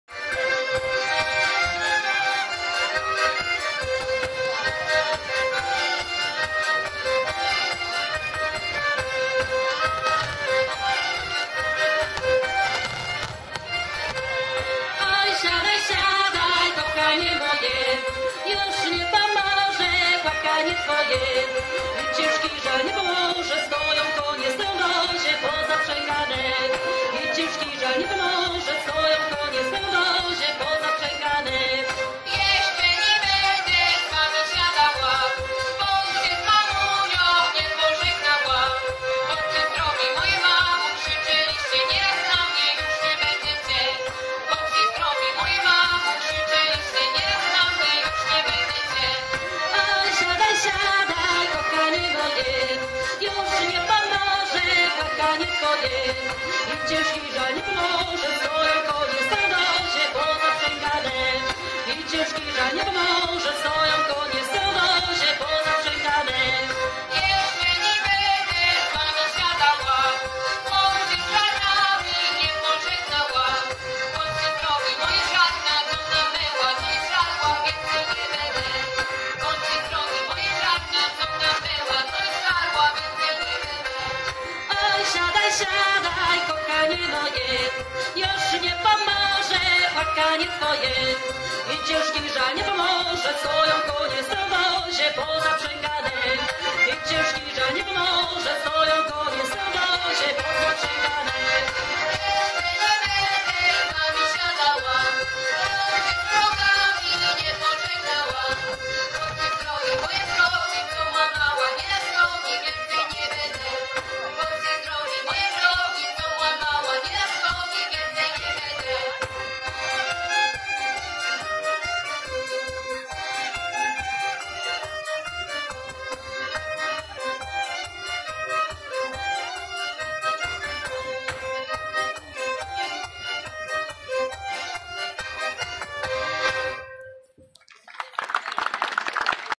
❤️ Pięknie gra i śpiewa zespół Galicjanki z Roztocza - występy z okazji II Dni Roztocza
Zespół "Galicjanki z Roztocza" z Narola występuje w muszli koncertowej w Tomaszowie Lubelskim z okazji II Dni Roztocza 2023 r. Tomaszów Lubelski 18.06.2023 r.
*Jeśli podoba Ci się ten film, to  kliknij łapkę w górę*

30618_182612